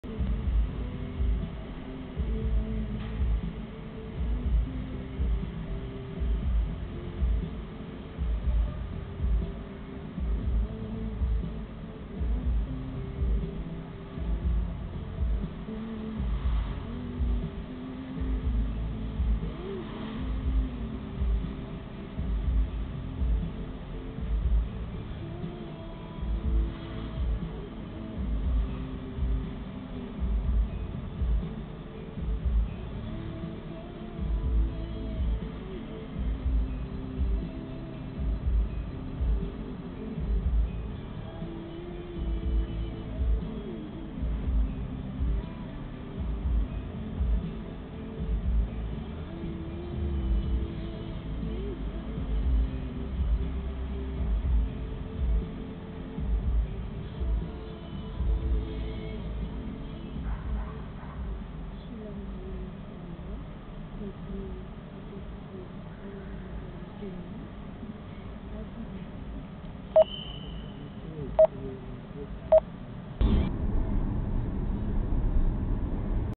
จอดขวางต้องระวัง
สังเกตุผู้หญิงที่อยู่ข้างหน้ารถว่าทำอะไรหลุดจากตัวรถ 555+